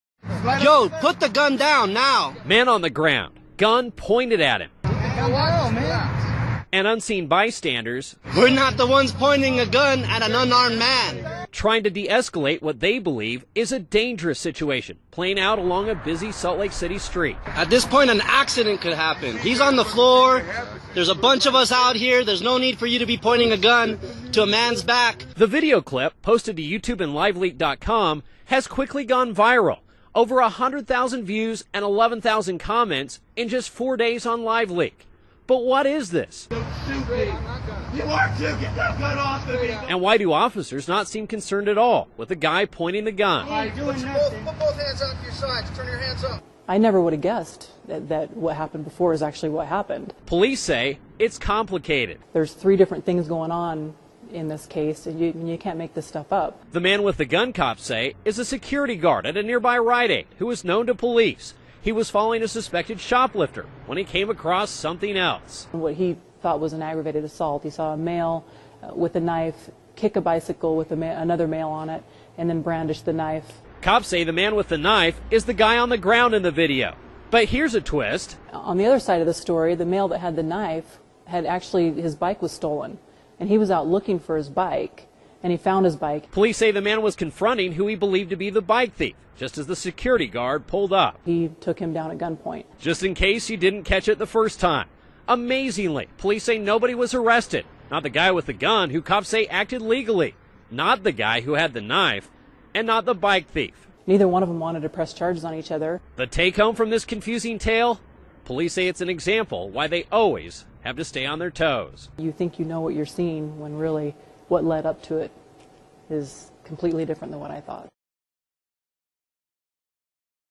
Man detains suspect at gunpoint
SALT LAKE CITY (CNN Newsource) -- A viral video shows a man holding another man at gunpoint on a Salt Lake City street.

When police arrive, they don't even seem to react to the gunman. Officers say the whole thing proves things aren't always the way they appear at first.

Andrew Adams walks us through it.